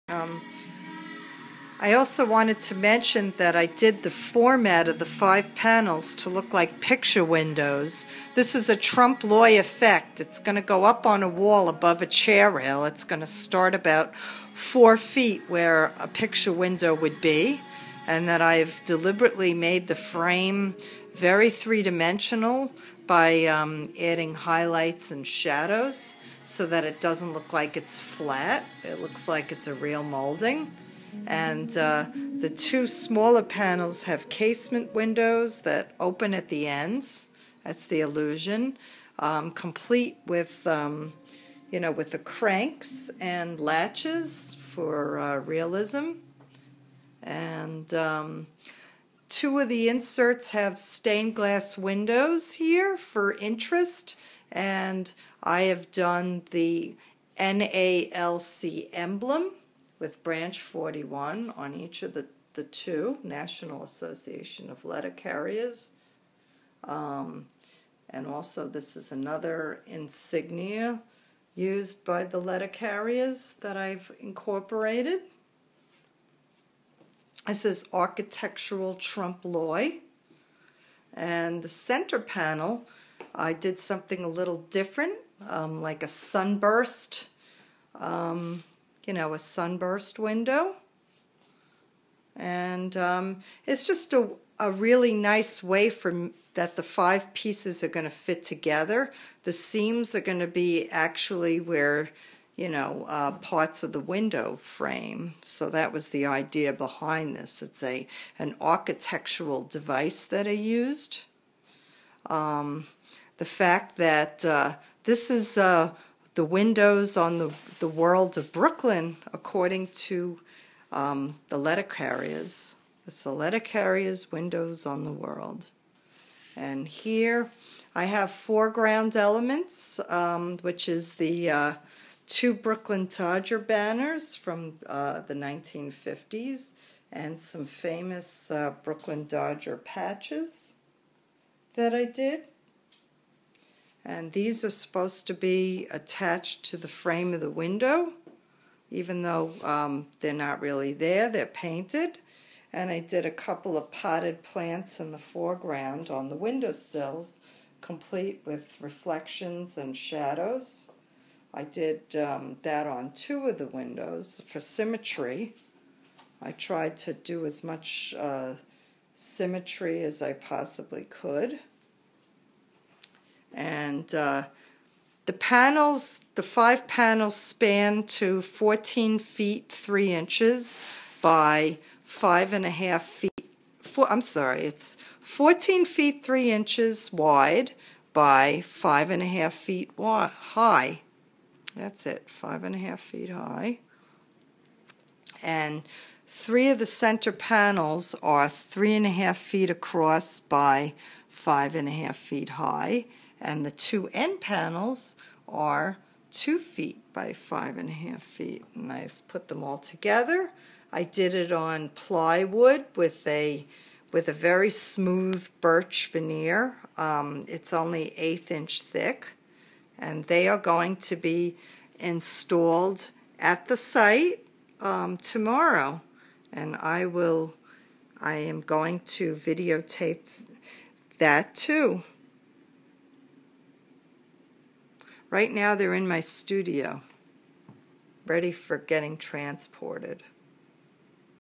Mural for National Association Of Letter Carriers Branch 41
Description of the troupe-l'oeil picture window and frames.